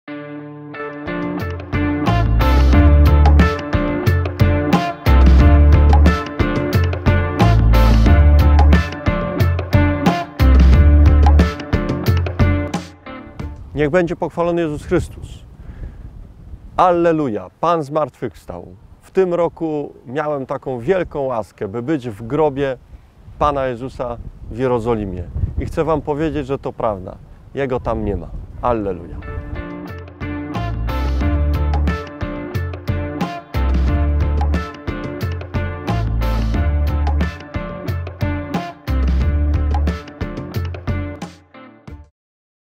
Na Fali Słowa - ks. Michał Olszewski SCJ - Niedziela Wielkanocna
Pierwszego dnia po szabacie, wczesnym rankiem, gdy jeszcze było ciemno, Maria Magdalena udała się do grobu i zobaczyła kamień odsunięty od grobu.